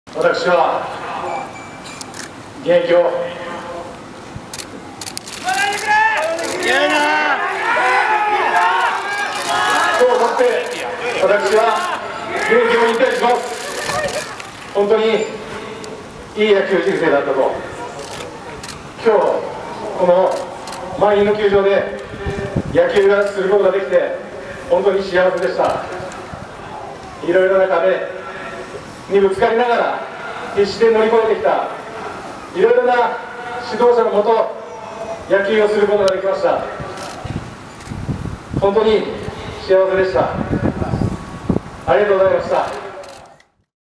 元巨人・村田、涙の引退表明 満員観客に「幸せでした」
プロ野球・横浜（現ＤｅＮＡ）や巨人で活躍し、今季は独立リーグのＢＣリーグ栃木に所属する村田修一選手が９月９日、栃木県小山市であった今季最終戦の群馬戦のあと、現役引退を正式に表明した。